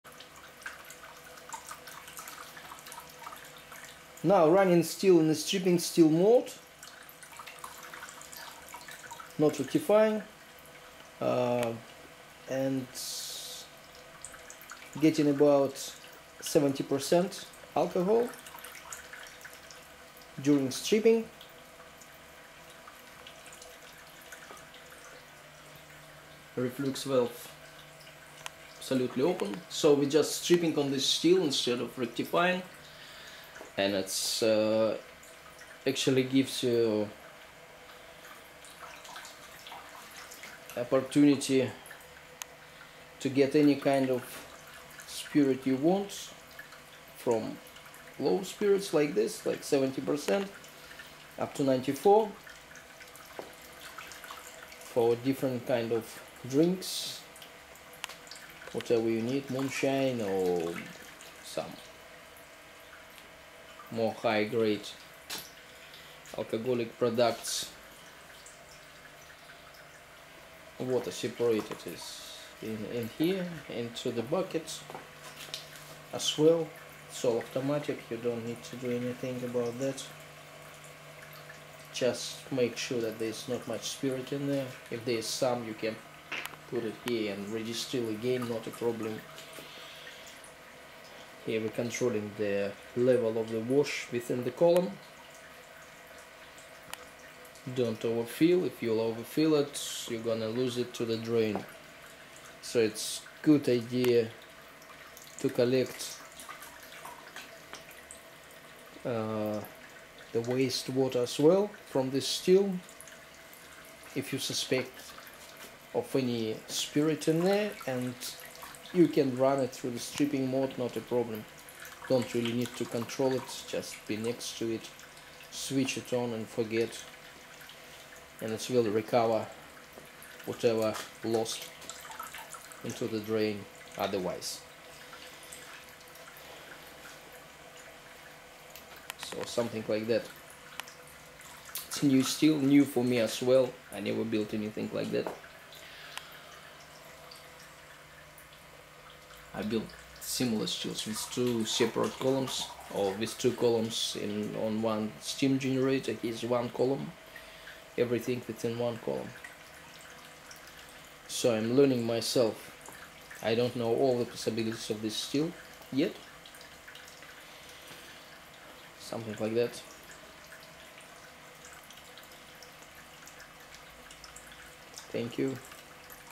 Continuous alcohol Reflux still, - "Primary distillation mode".
Continuous Reflux still, testing in a straight distillation - no reflux, just continuous primary distillation.